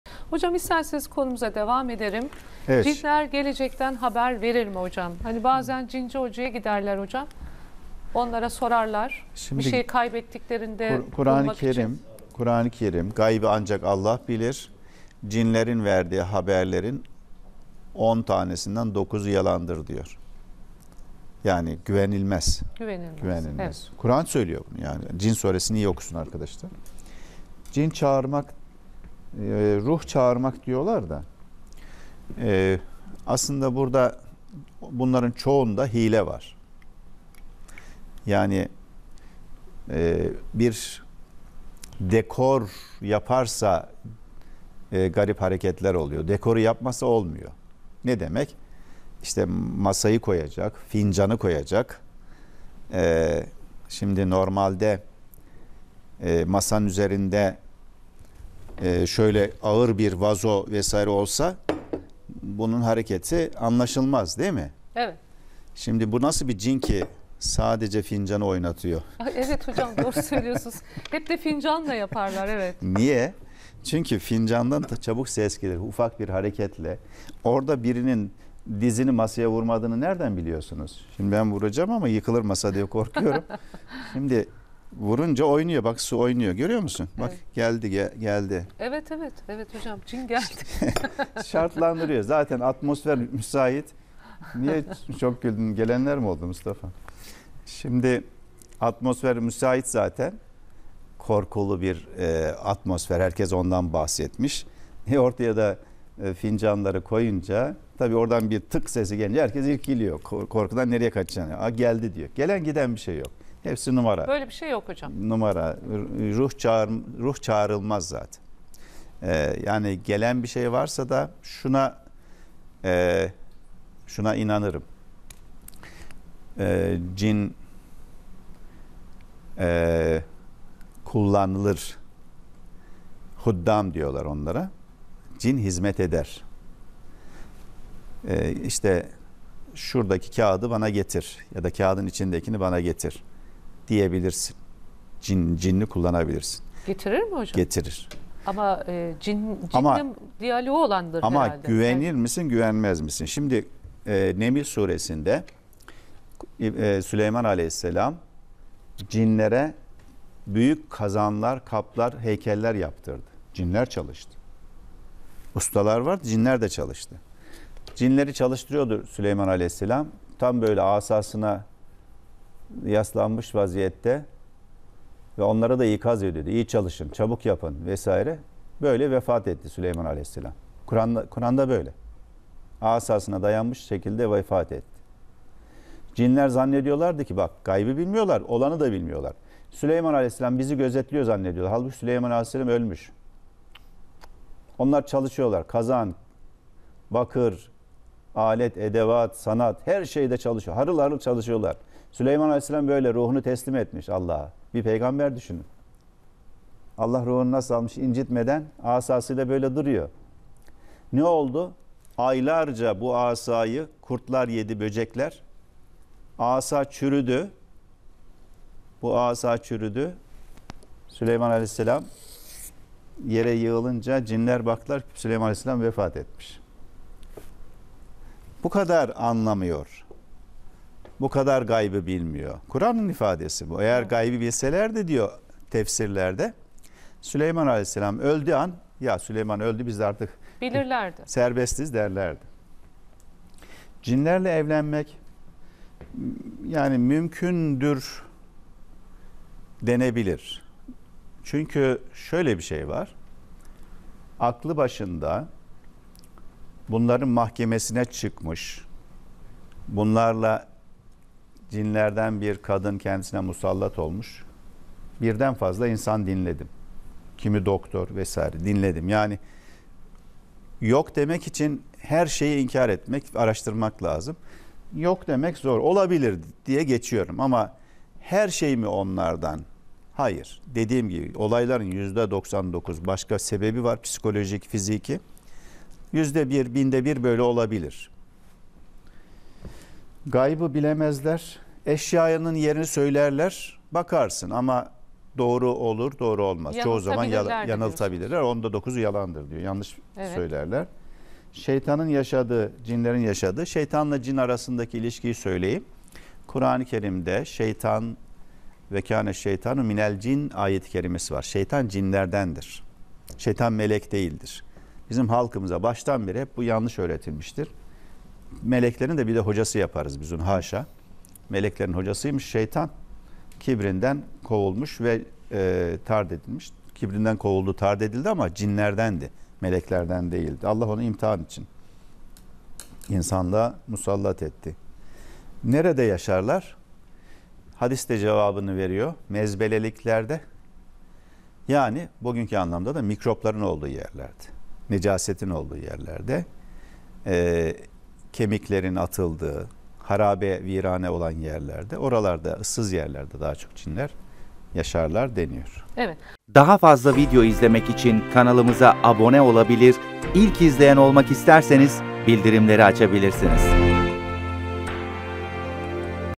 Süleyman(a.s) Kıssası Cinlerle İlgili Neyi Anlatır? | Prof. Dr. Mustafa Karataş ile Muhabbet Kapısı
Cinler geleceği bilebilir mi? Huddam cinleri gerçek midir? Süleyman Peygamber kıssasında cinlerle ilgili neyi anlamalıyız?